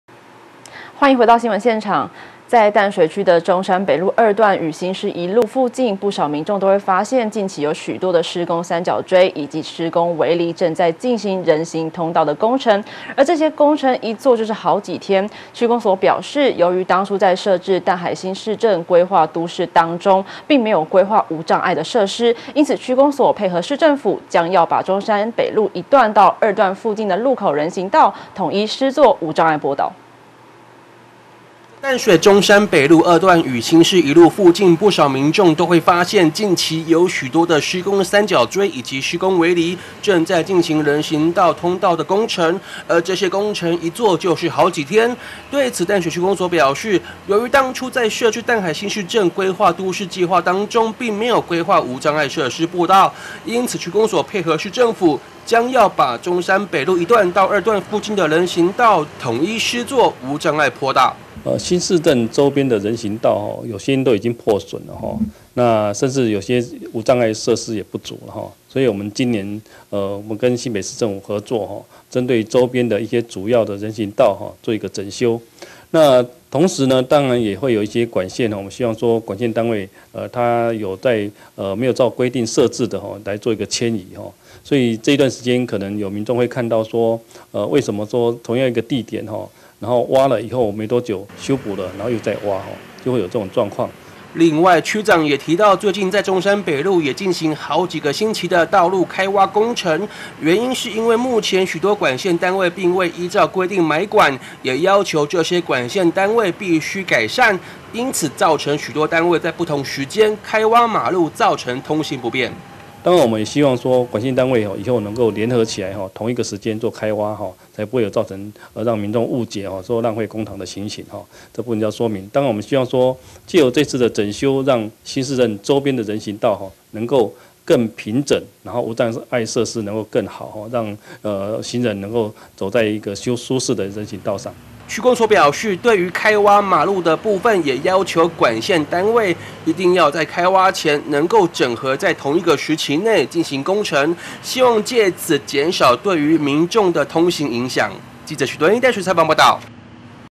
1070420新北新聞網06 淡水區新市鎮人行道大翻修 增設無障礙設施
在淡水區中山北路二段，與新市一路附近，不少民眾都會發現，近期有許多的施工三角錐以及施工圍籬正在進行人行通道的工程，而這些工程一作就是好幾天，區公所表示，由於當初在設置淡海新市鎮規劃都市當中，並沒有規畫，無障礙設施，因此區公所配合市政府，將要把中山北路一段到二段附近路口的人行道，統一施作無障礙坡道。